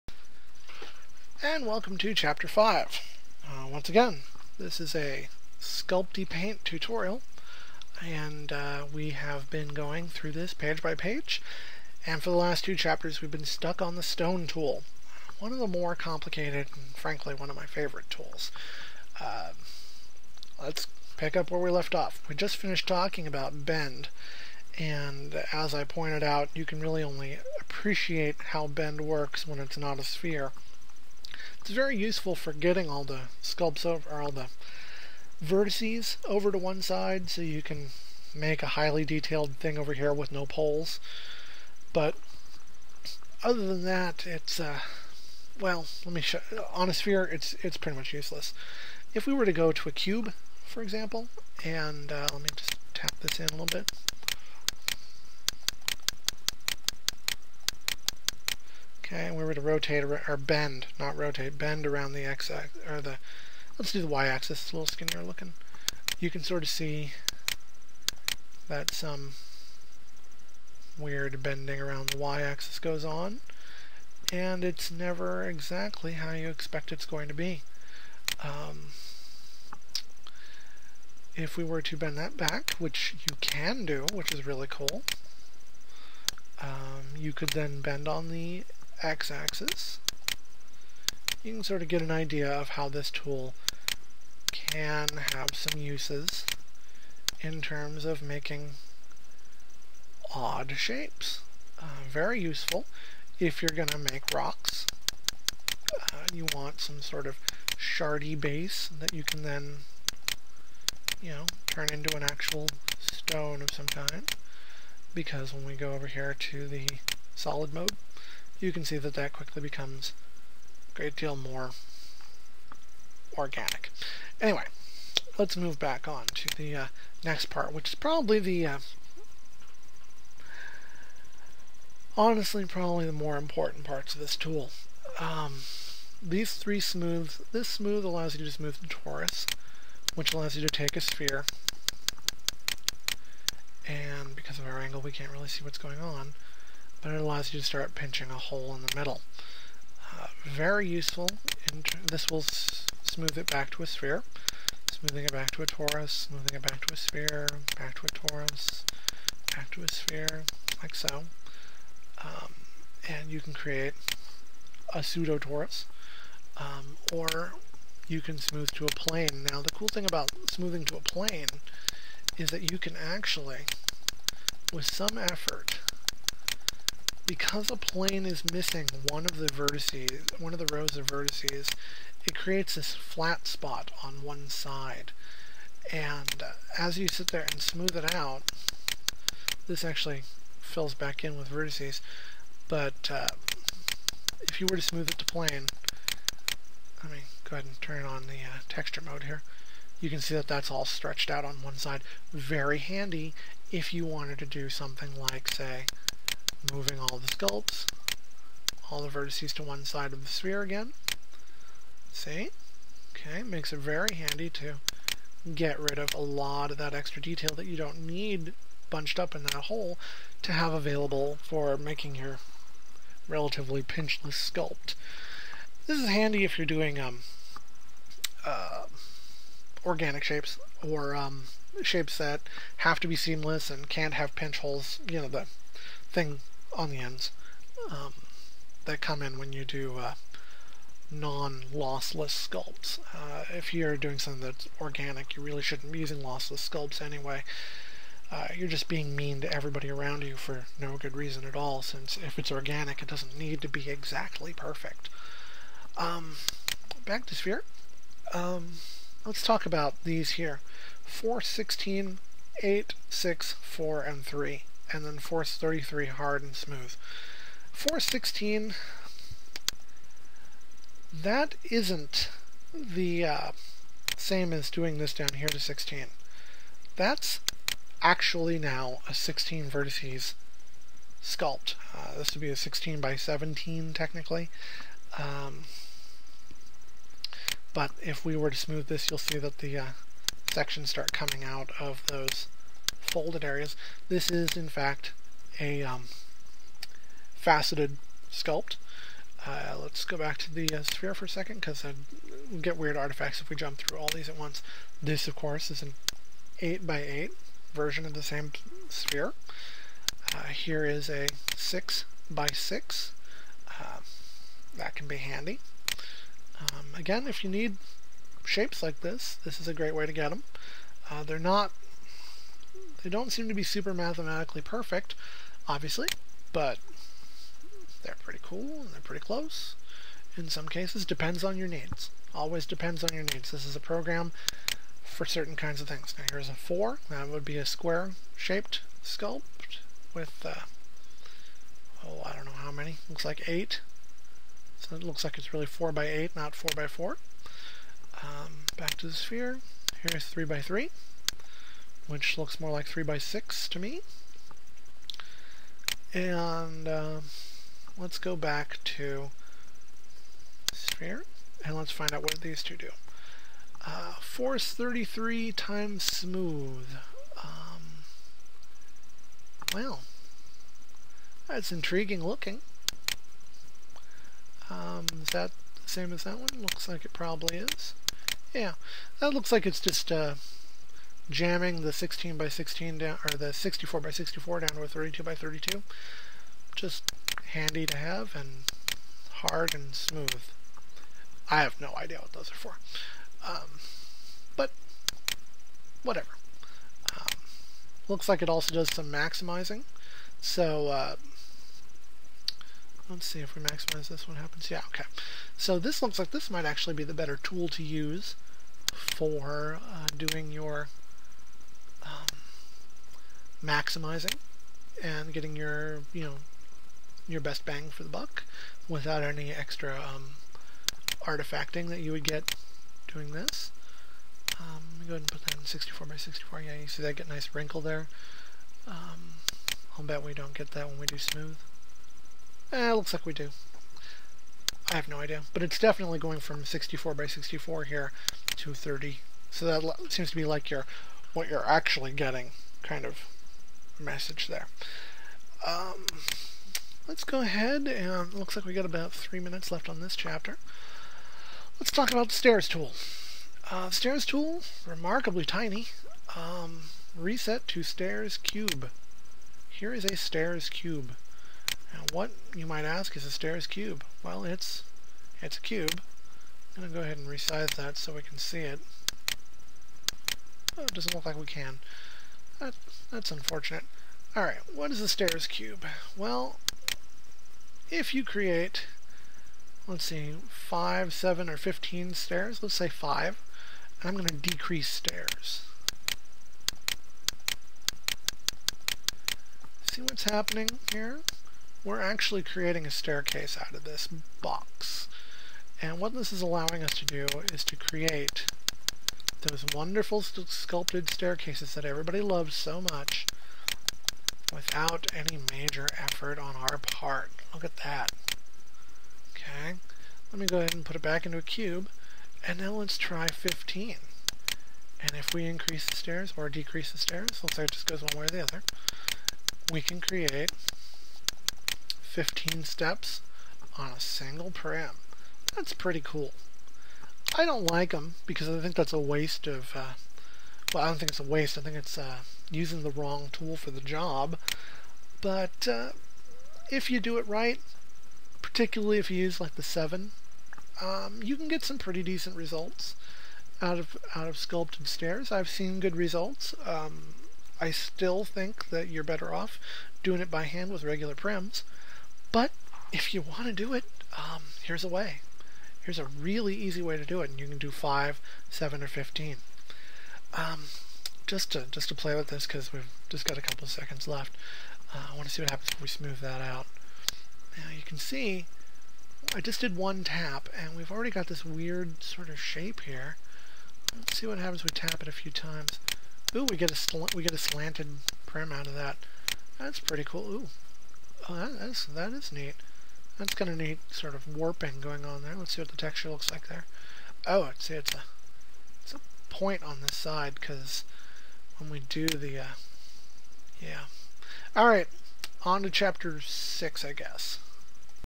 Winter's SculptyPaint Tutorial (5of9) "Stone Tool (pt 3) & Stairs Tool"
A software tutorial for SculptyPaint v.092 (created by Cel Edman & Pixel Lab). Chapter 5 (of 9) rounds out the 3-part mini-lesson on the "Stone Tool", covering the "Force to resolution" buttons, and gives a cursory tour of the "Stairs Tool"

Support Cel Edman by buying some of his premade sculpty packs on XStreetSL!